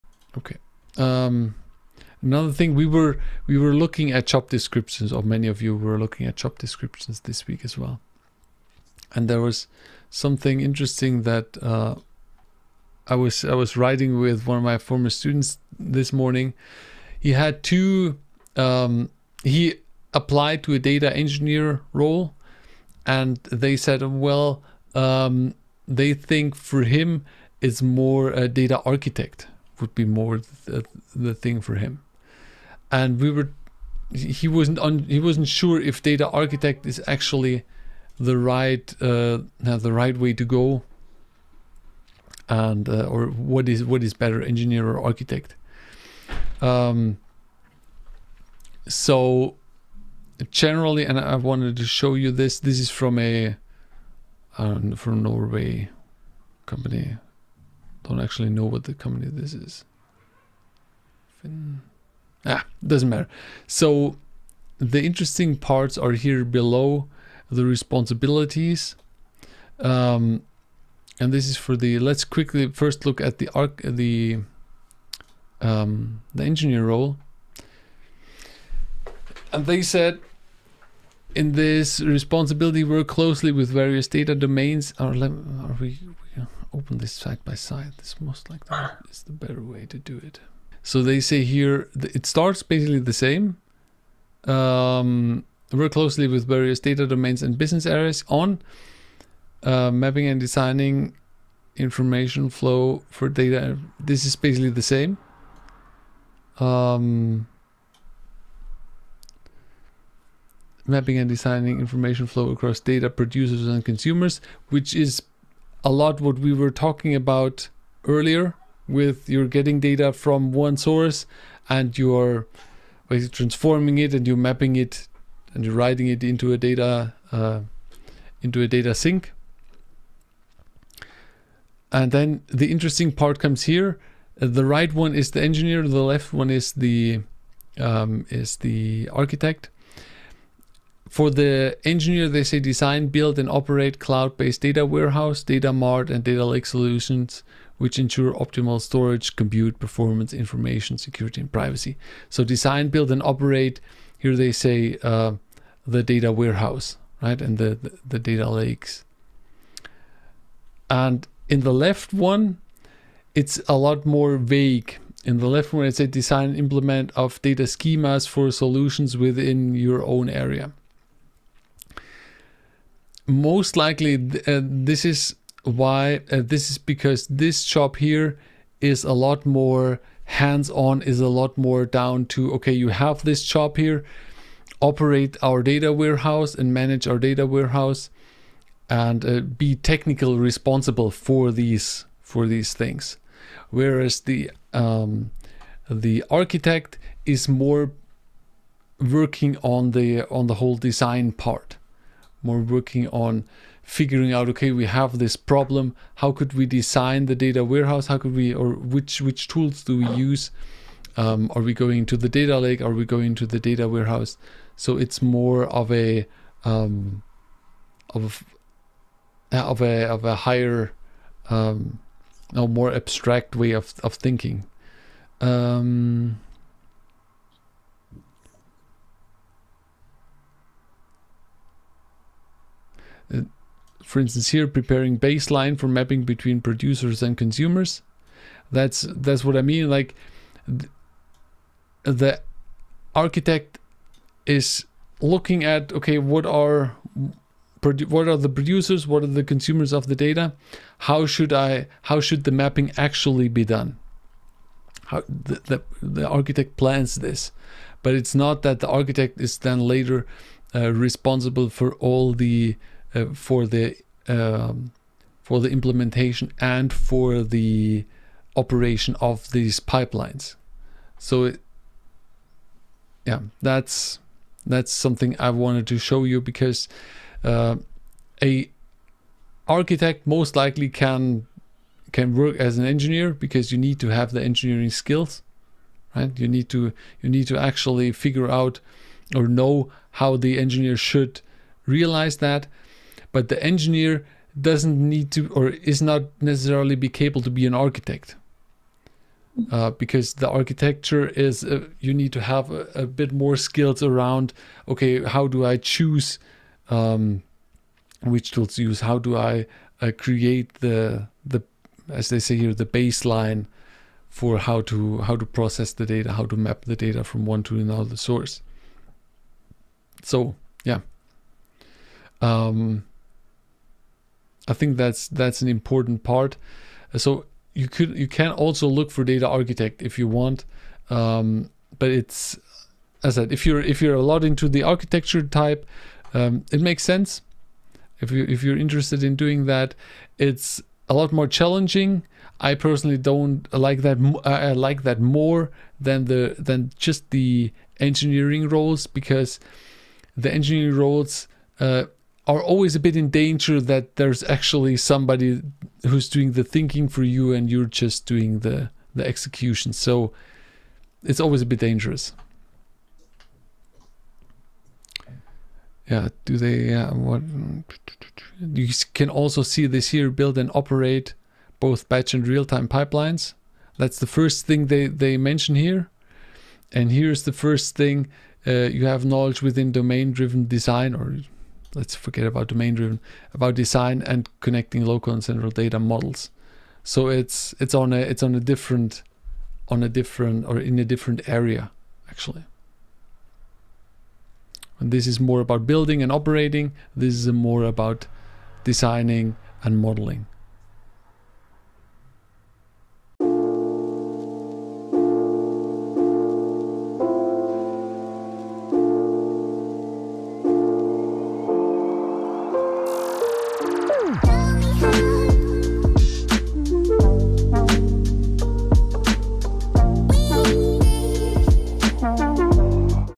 Job Analysis: Data Architect or Data Engineer which one is best?
This is a video of my weekly Data Engineering Coaching meeting. I showed two job descriptions: One for a Data Architect and one for Data Engineer. One of my students were asking during the week which one is better.  There's no real better. As the name suggests, the Architect is more concerned with the planning of pipelines, data models and the platform. The Engineer is more hands on and has a lot to do with development and operations.